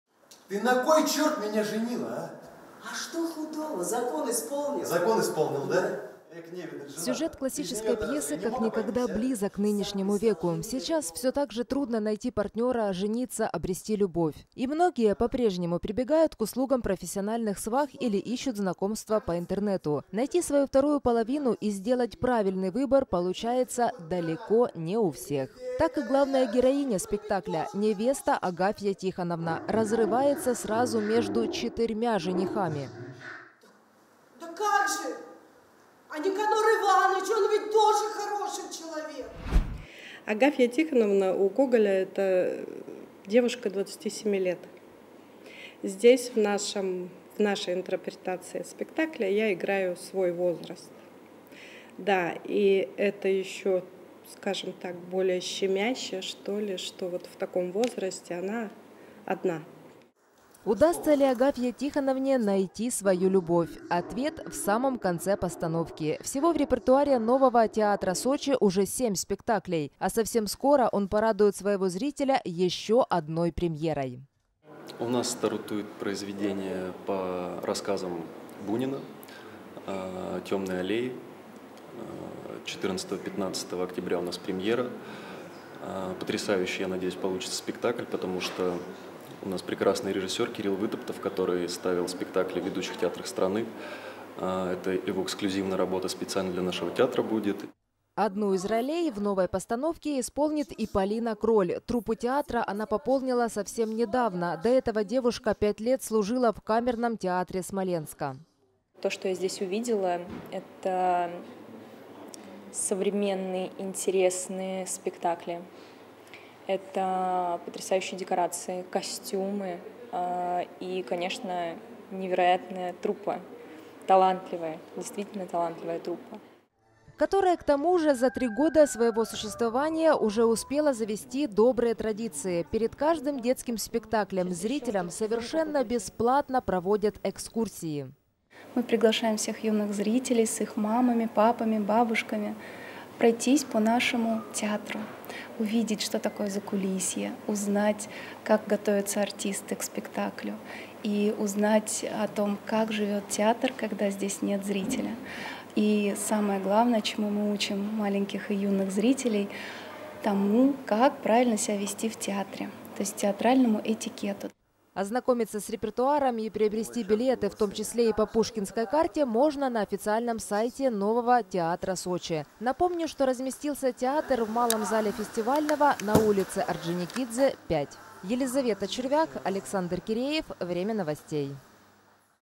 25-го сентября в "Новом театре Сочи" стартует очередной сезон.
Откроет его спектакль "Женитьба". Герои одной из самых известных комедий Гоголя обретают современное звучание и оживают в новых реалиях 21-го века. Елизавета Червяк расскажет подробнее.